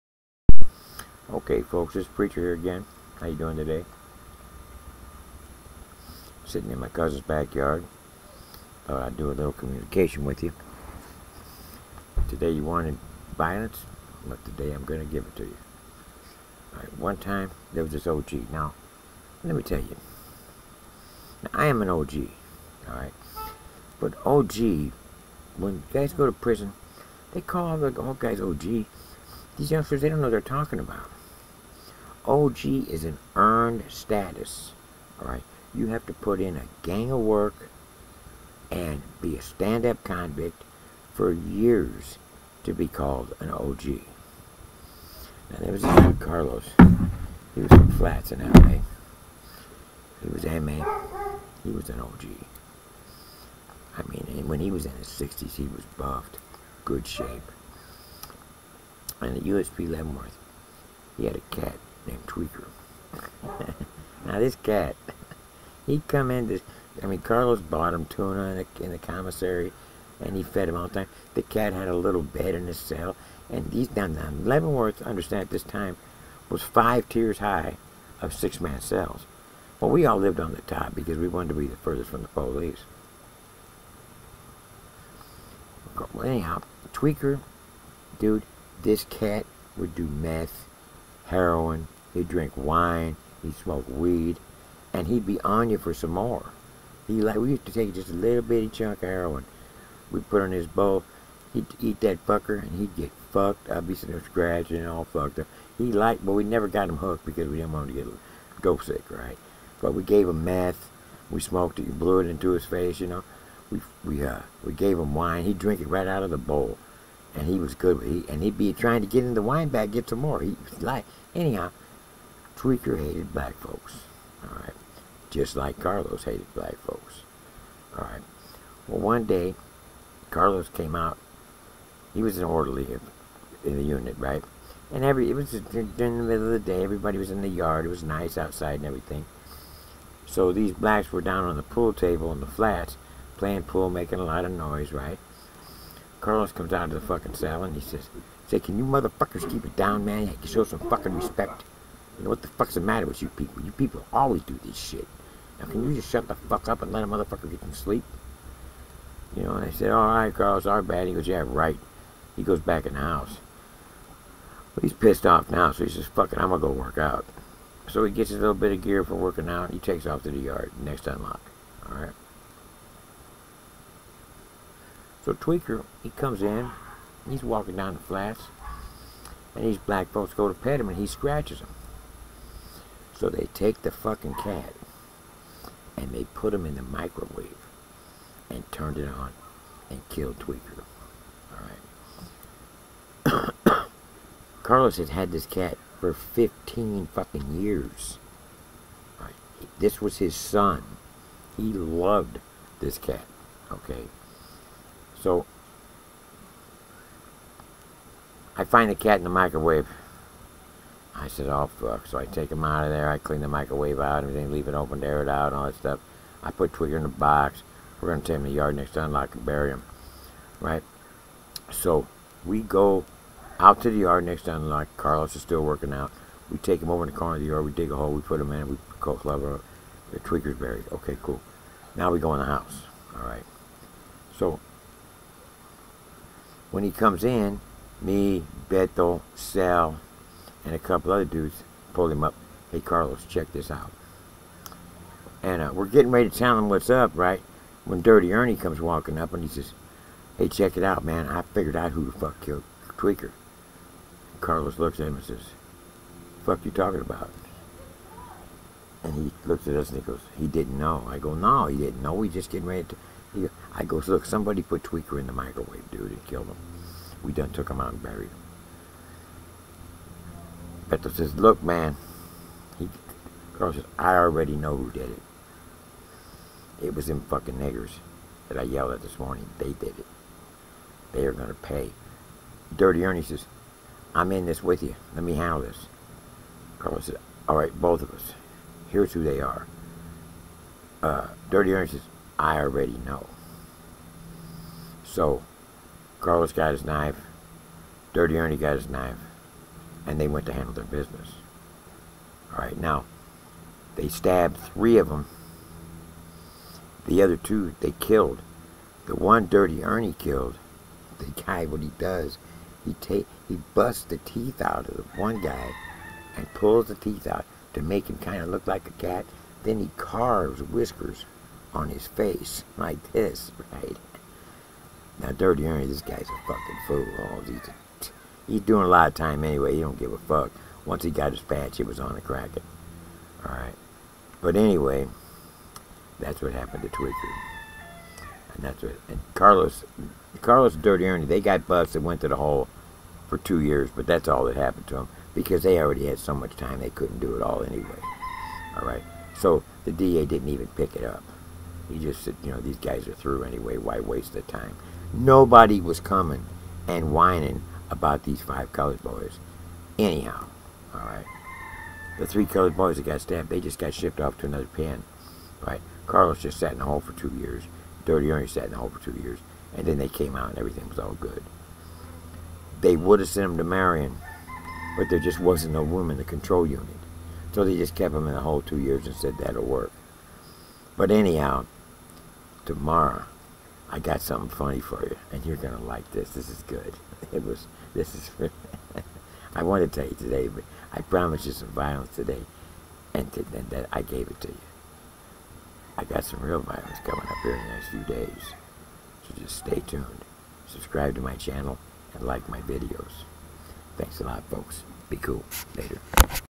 Tweaker the cat
This is about a gut and his cat. This guy loved his cat like a son and when the cat was murdered he took it to the next level.